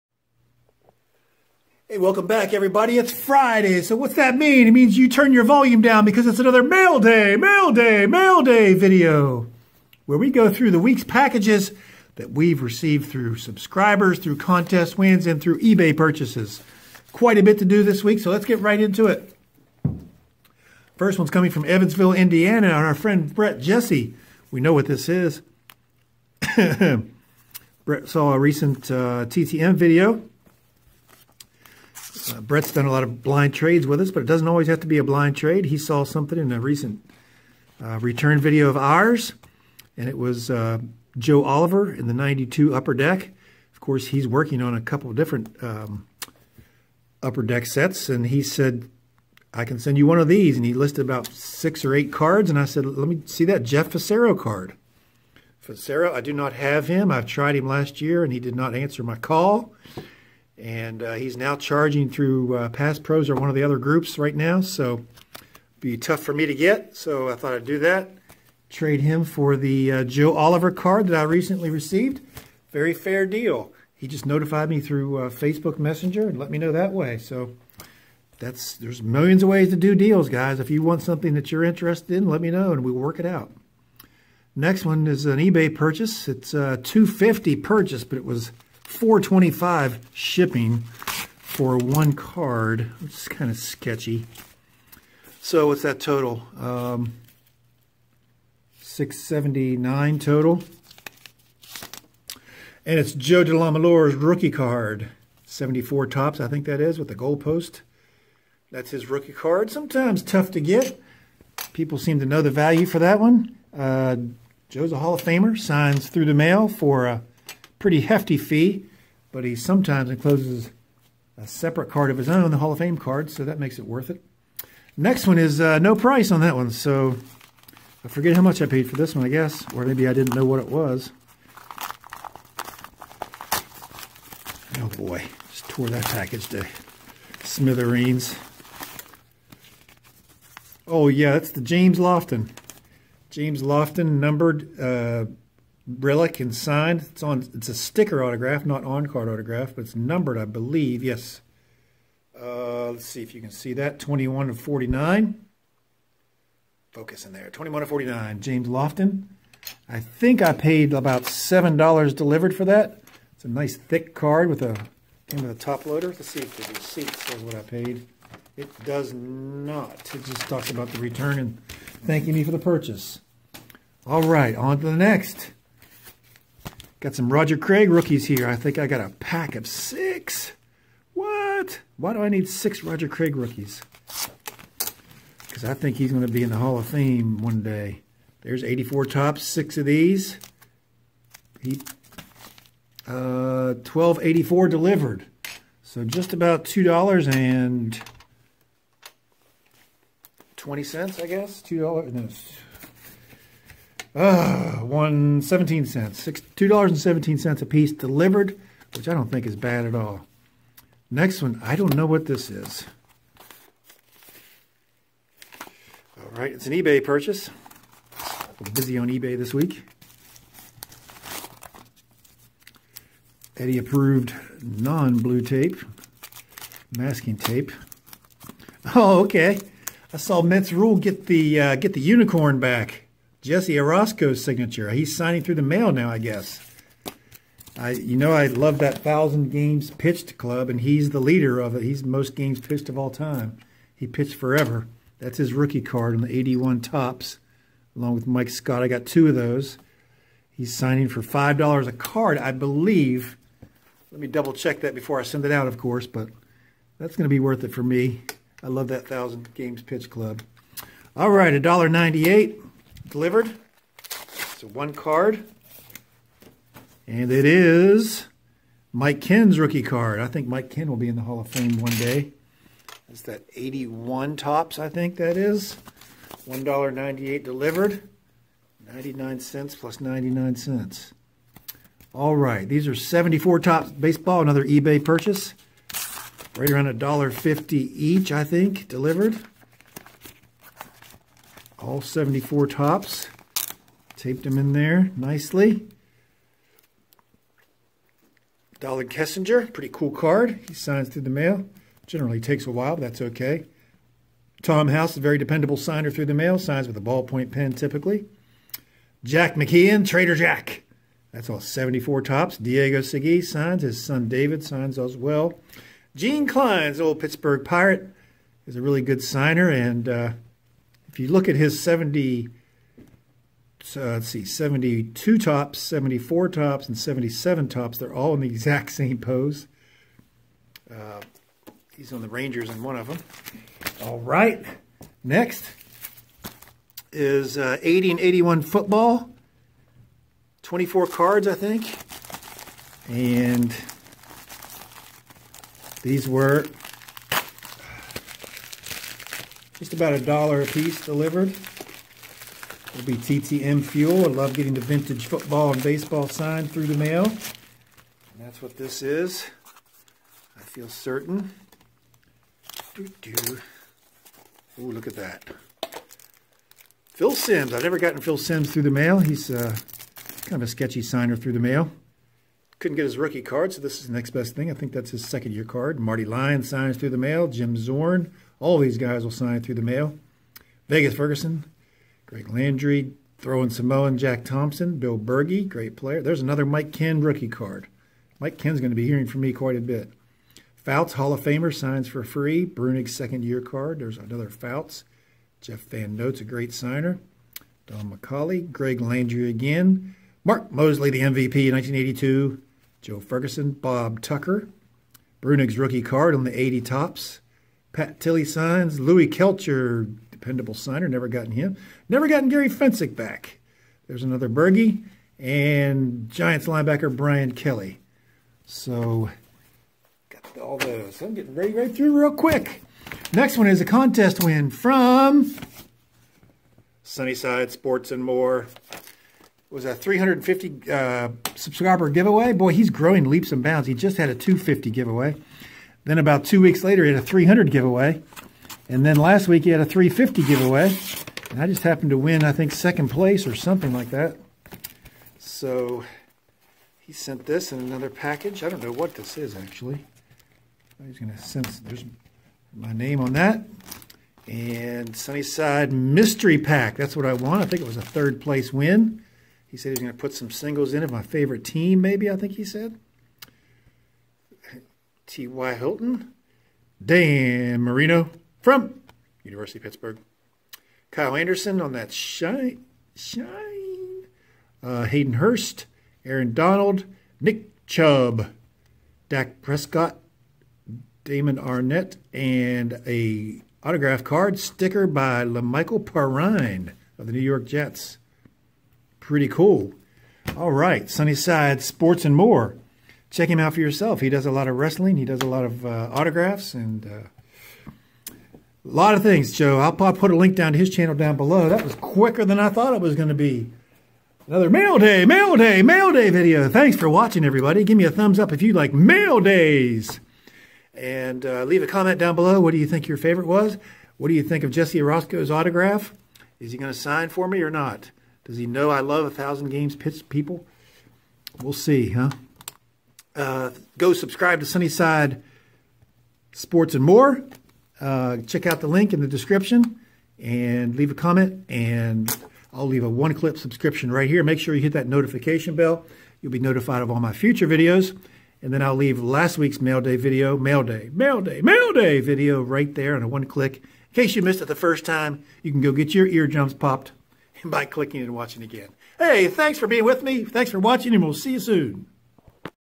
Mail Day! Mail Day! Tons of TTM Fuel! Sunnyside!
In this Mail Day video, we open all the packages we have received during a busy week of mail -- eBay purchases, a pre-arranged trade with a YouTube subscriber, and a contest win from fellow Youtuber 'Sunnyside Sports and More'!